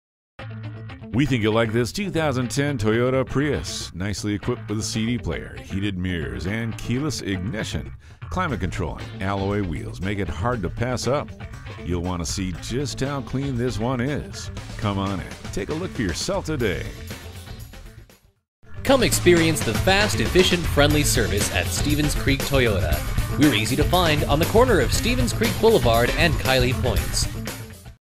2010 Toyota Prius San-Jose CA Burbank, CA CT23388 - SOLD
Year: 2010
Make: Toyota
Model: Prius
Trim: Ii Hatchback
Engine: 4 Cyl. 1.8L
Transmission: Variable
Color: Classic Silver Metallic
Interior: [none]
Mileage: 28853
Stock #: CT23388

Address:
4202 Stevens Creek Blvd